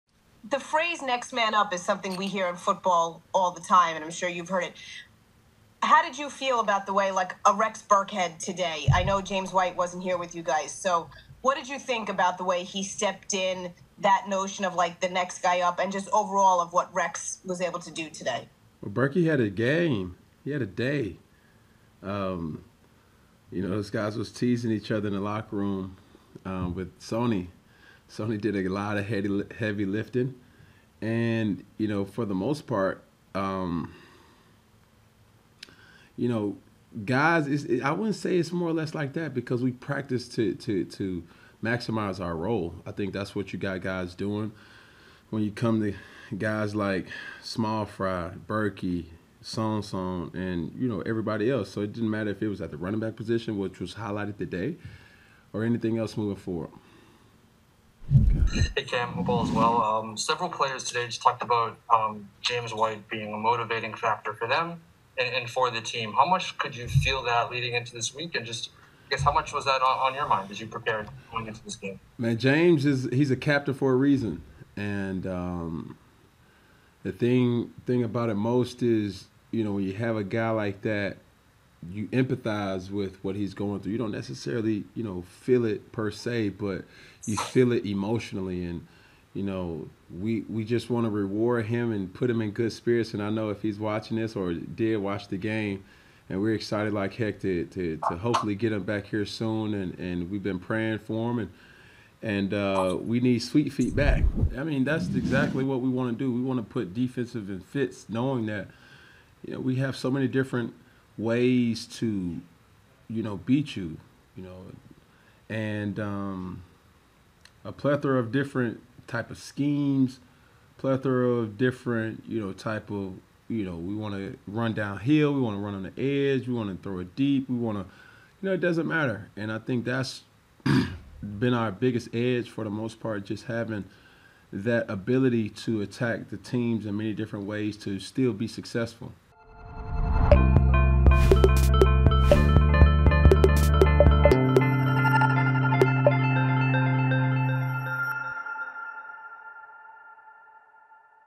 Cam Newton Postgame Press Conference | Patriots vs Raiders | Week 3
FOXBOROUGH--  Cam Newton discusses the absence of James White and the teams motivation to play for him.  Cam also addresses his impression of the Patriots team so far.